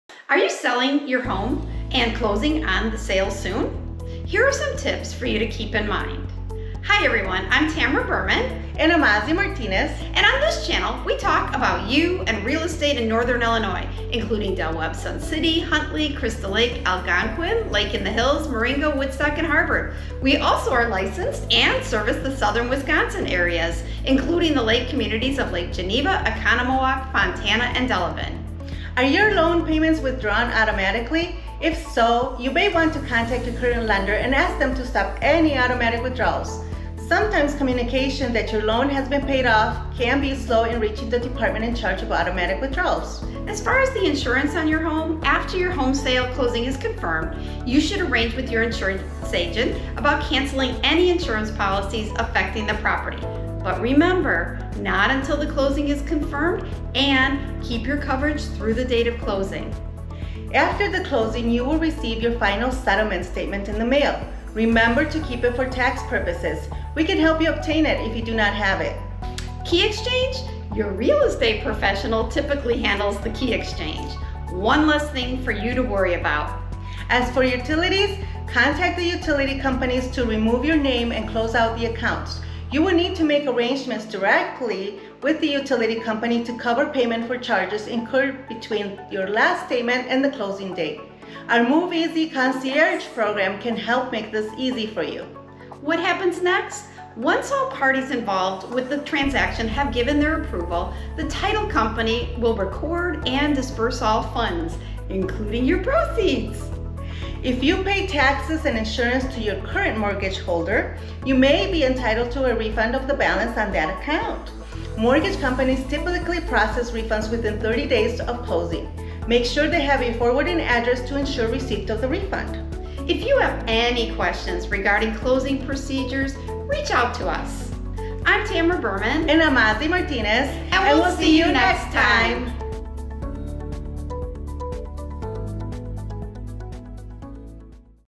Selling your Home In the Huntley IL Area - 7 Key Reminders to Save You Time, Money, & Aggravation.
Are You Selling Your Home And Closing On The sale Soon?
Here are some reminders for you to keep in mind.

1. ARE YOUR LOAN PAYMENTS WITHDRAWN AUTOMATICALLY?
Ask your current lender to stop any automatic withdrawals.  Sometimes communication that your loan has been paid off can be slow in reaching the department in charge of automatic withdrawals.

2. HAZARD INSURANCE FIRE, FLOOD, ETC.)
After your home sale closing is confirmed, cancel any insurance policies affecting the property.

3. FINAL SETTLEMENT STATEMENT
After closing you will receive your final settlement statement in the mail.
Remember to keep it for tax purposes

4. KEY EXCHANGE
Your real estate professional typically handles the key exchange. One less thing for you to worry about.

5. UTILITIES
Contact the utility companies to remove your name and close out the accounts. The final utility bill may not be paid through escrow; you will need to make arrangements directly with the utility company to cover payment for charges incurred between your last statement and the closing date.

6. WHAT HAPPENS NEXT?
Once all parties involved with the transaction have given their approval, the title company will record and disburse all funds, including your proceeds.

7. ESCROW ACCOUNT
If you pay taxes and insurance to your current mortgage holder you may be entitled to a refund of the balance on that account. Mortgage companies typically process refunds within 30 day of closing Make sure they have your forwarding address to ensure receipt of the refund.

Tamara & Ozzie Team
Real People Realty

Serving the Nothern Illinois & Southern Wisconsin Areas – finding the best real estate solutions and making buying and selling homes a smooth experience.